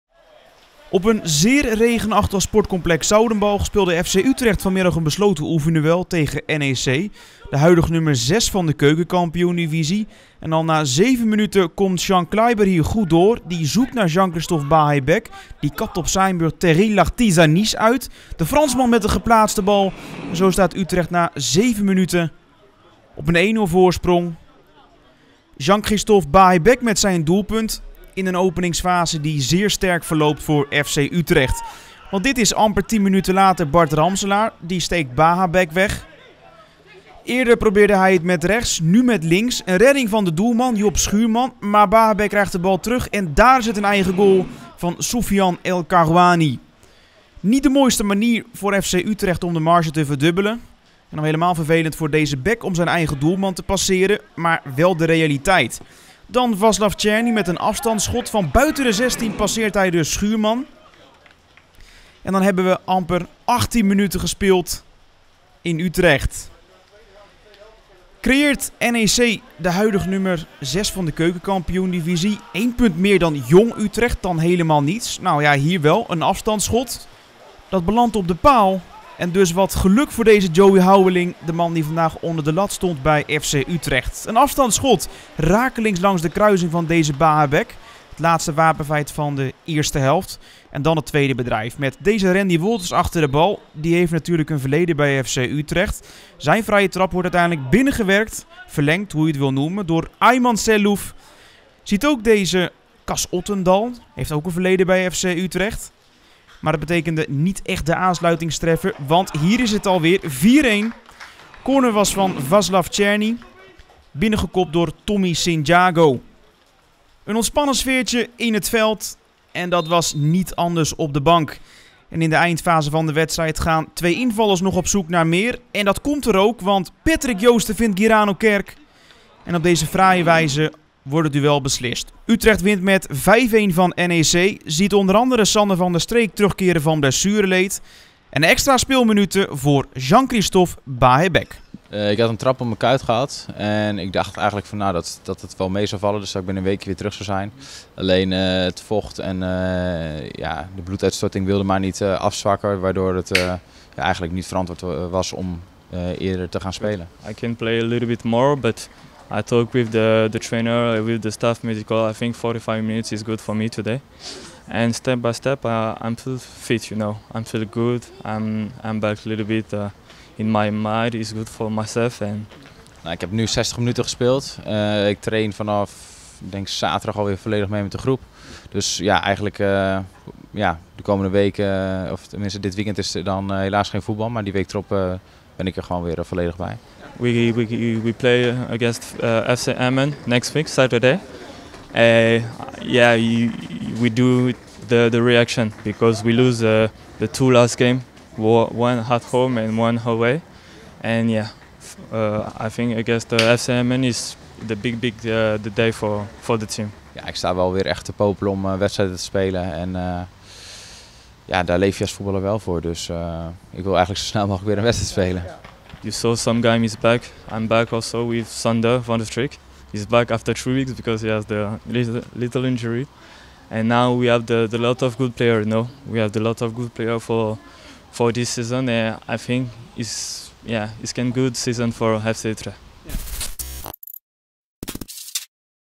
HIGHLIGHTS | FC Utrecht - NEC
In een vriendschappelijke wedstrijd achter gesloten deuren was FC Utrecht vanmiddag (woensdag) een maatje te groot voor NEC. De Utrechtse Eredivisionist won met 5-1 van de Nijmeegse Keuken Kampioen Divisie-ploeg. Er zaten prachtige goals tussen; bekijk de samenvatting maar!